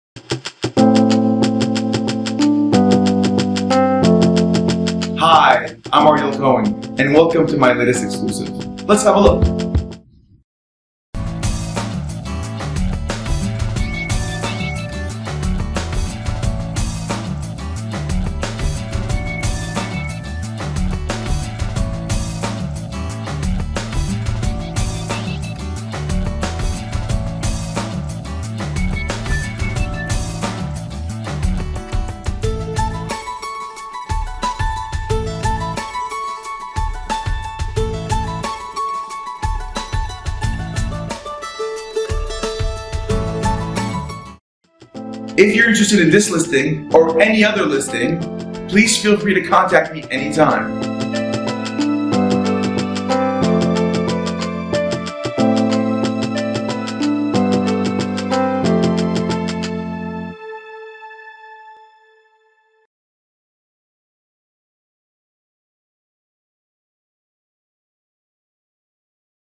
Financial District, 75 Wall Street, PHD1 - Ariel Cohen - 07/18/13 - 1584124
Financial District, 75 Wall Street, PHD1

75 Wall Street defines world class luxury living with a full-service lifestyle and an abundance of amenities.  Well appointed luxury condominiums offering dramatic 360 degree views of downtown Manhattan comprise the upper 24 floors of 75 Wall.  75 Wall residents have access to integrated hotel services including room service, catering, housekeeping, and valet laundry, as well as a full-time personal concierge.Designed by Rockwell Group, this 4 bedroom 4 and a half bathroom residences feature ten-foot ceilings and oversized windows, in-residence washers and dryers, wide-planked cerused oak flooring, CasesarStone Countertops, and Lacquer Kitchen Cabinetry by the Italian Design Studio, Boffi.  Appliances by Sub-Zero, Liebherr, Bosch, Miele, Electrolux, and Sharp.  Bathrooms feature custom wood cabinetry, Botticino Semi Classico Marble Floors, and Tub Surround.